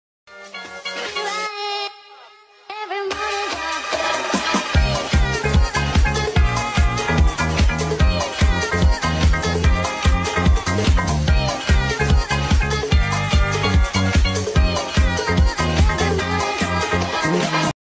sad cat dance meme
My character Anastasia does the Sad Cat Dance Meme lmao